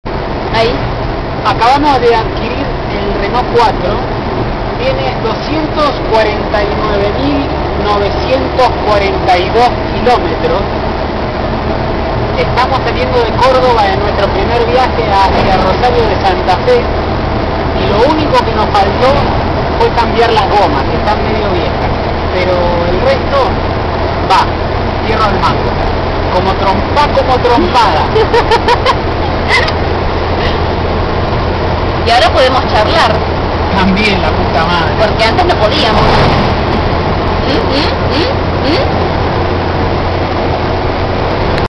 Renault 4 - Córdoba Rosario
Primer viaje del Jean sobre ruedas

Comportamiento exelente.
12km/lts a 90km/h